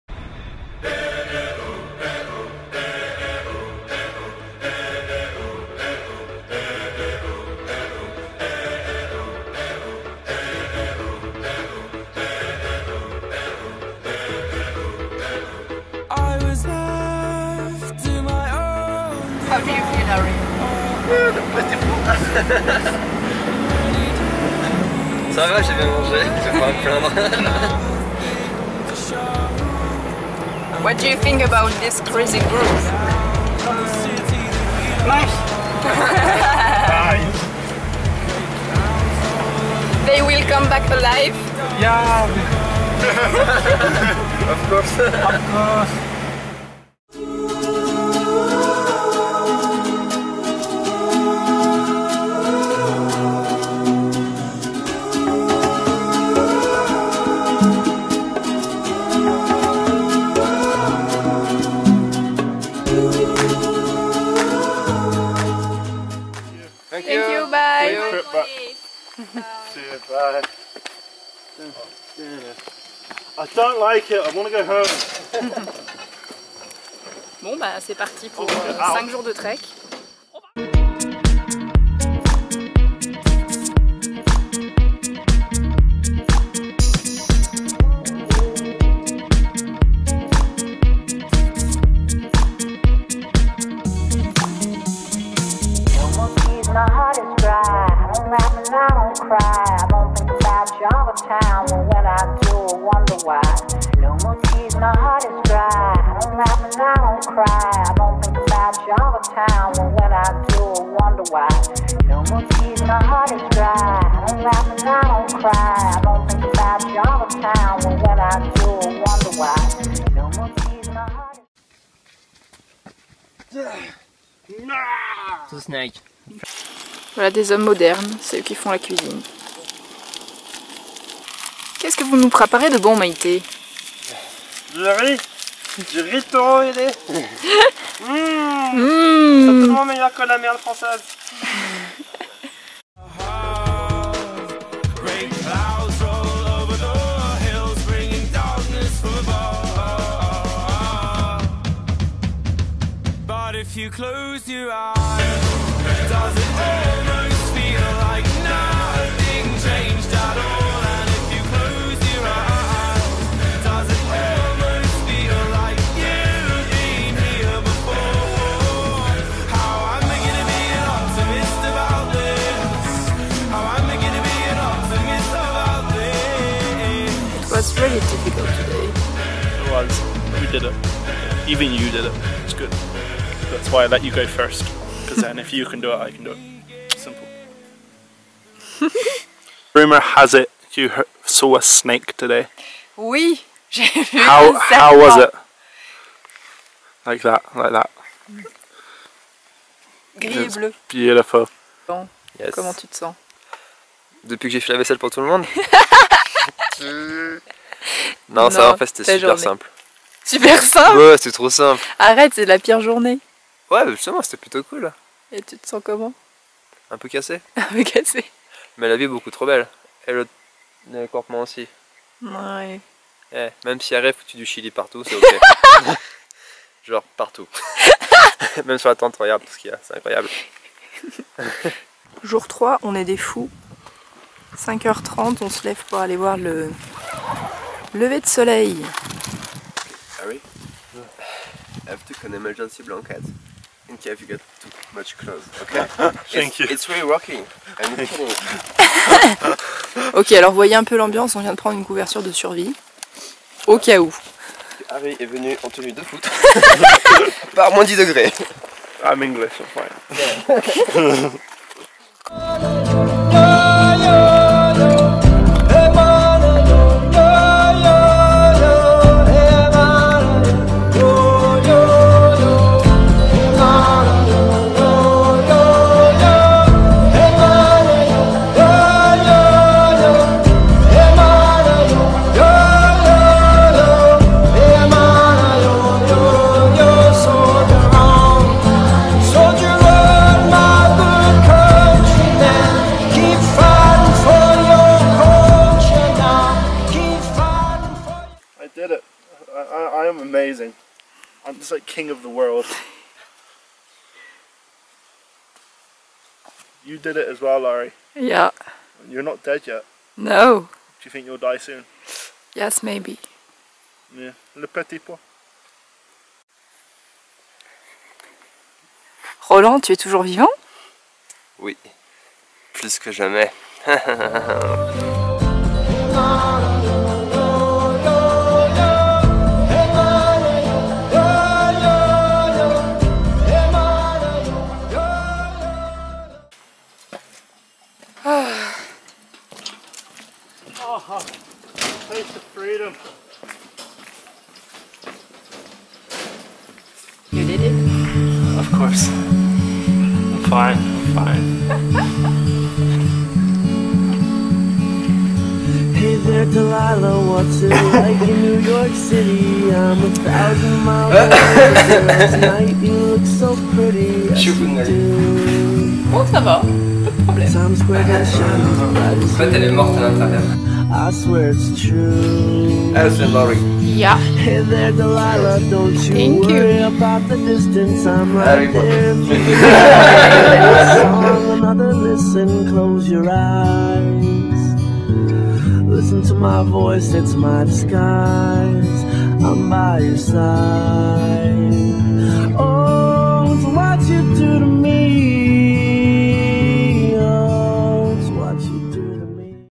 Gunung Tahan (Malaisie)
4 jours de trek en autonomie à la conquête du Gunung Tahan, plus haut sommet de la péninsule malaisienne. 2000 m de dénivelé positif.
2 jours d'ascension et 2 jours de redescente.
4 days self-reliance trekking to Gunung Tahan, the higher peak of malaysian peninsula. Positive difference in height of 2000 meters.
2 days climbing and 2 days of descent.